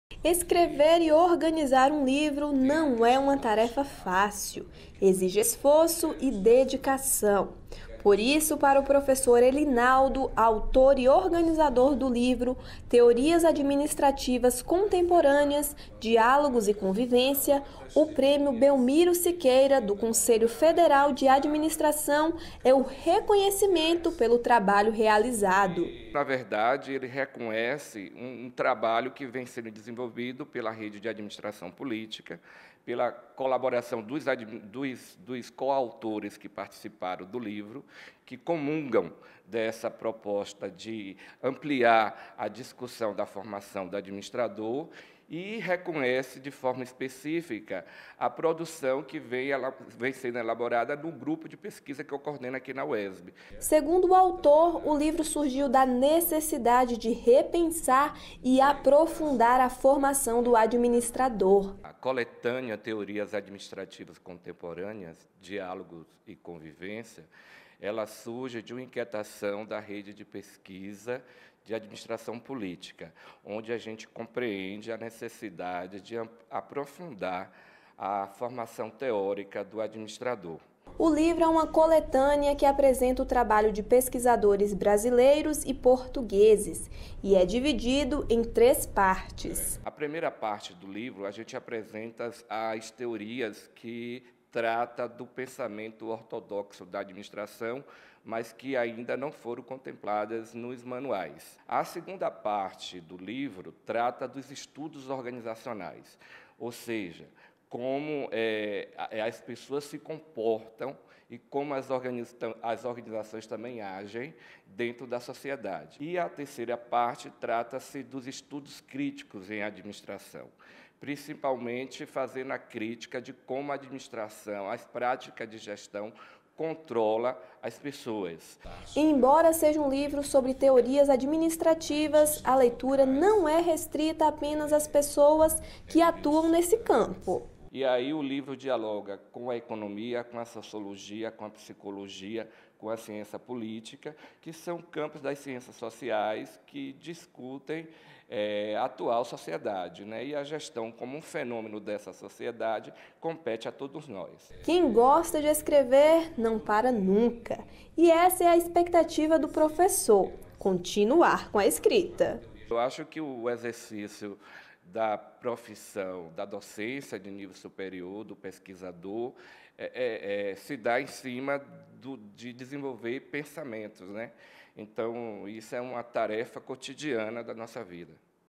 Matéria sobre o livro "Teorias Administrativas Contemporâneas" exibida em dezembro de 2017O
Imagens: Maurício Amaral
Reportagem: Andréia Giovanni
Elinaldo Leal Santos: professor e escritor